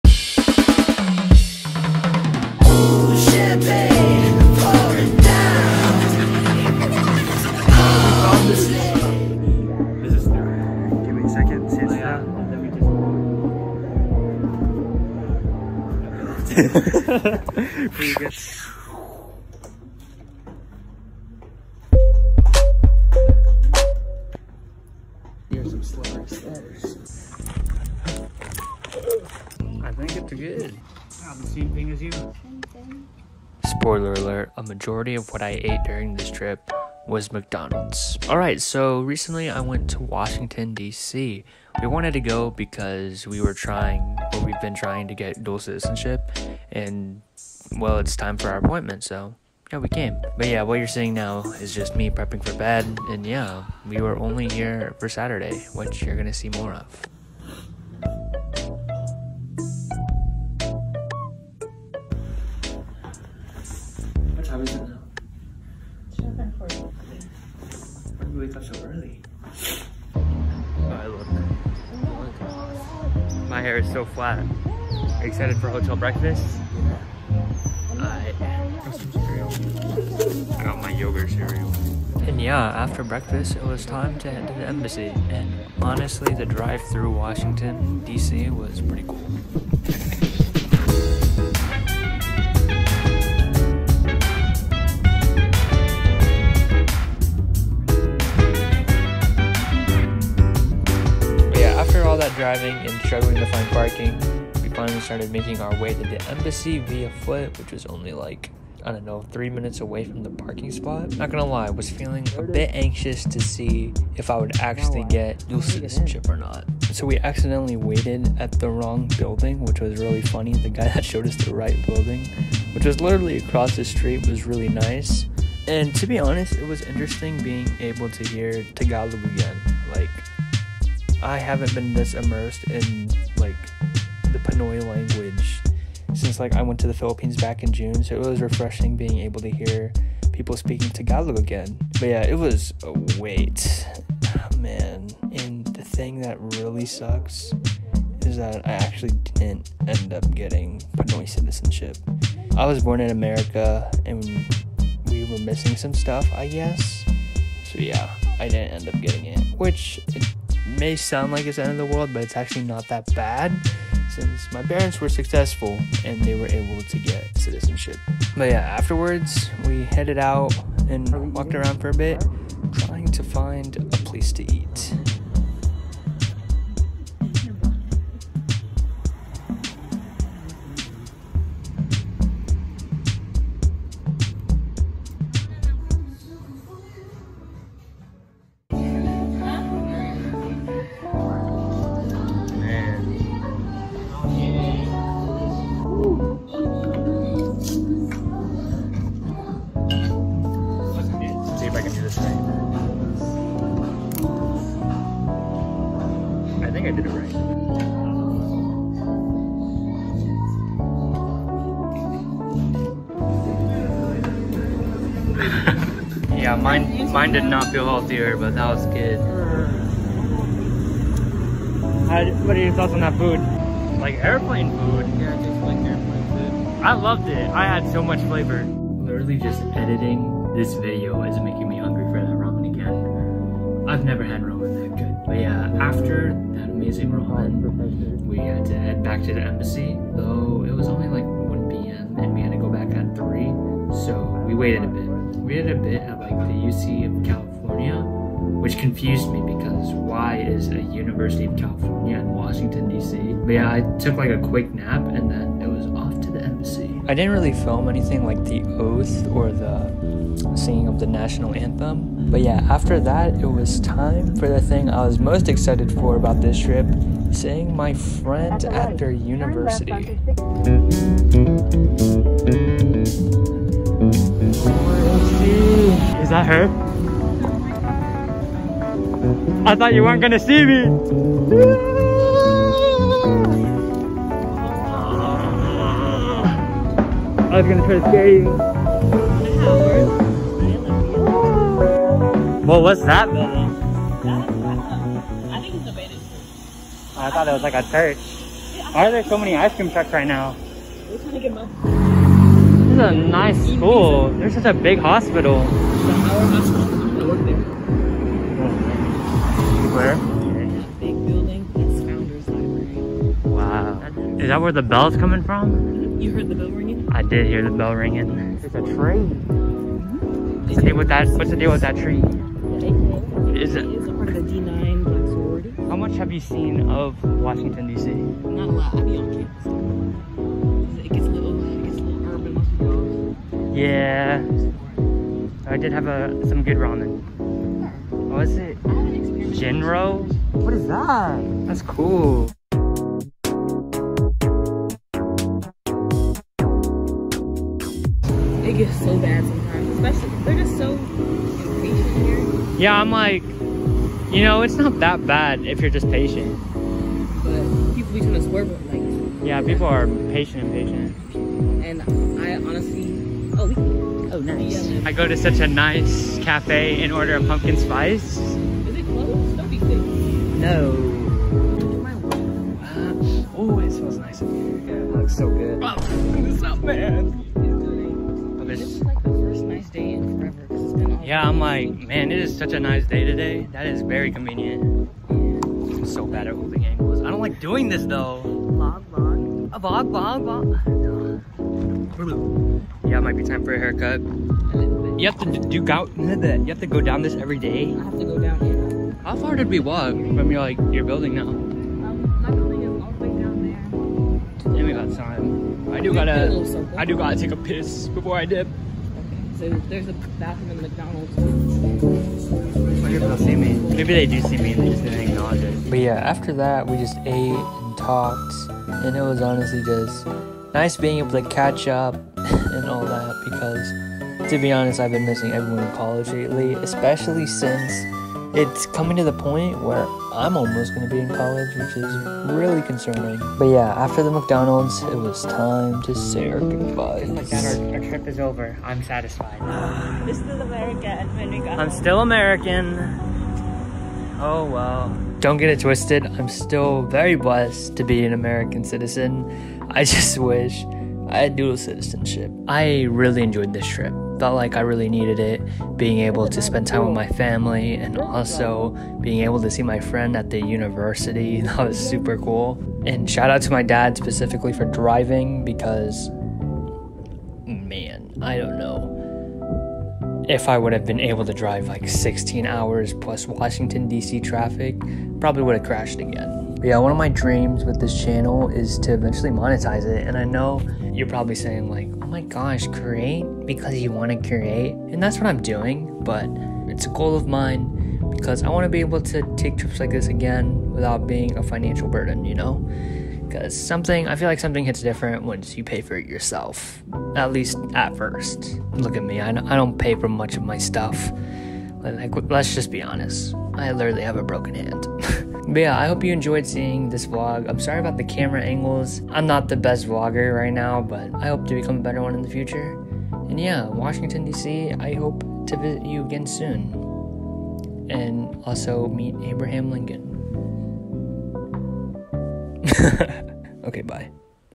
visiting washington dc for pinoy citizenship & seeing mah friend
revently me and my family went to washington dc to try to get filipino citizenship. along the way I realized that my friend's university was a couple minutes away so ye that's the video :D

thanks again for watching :D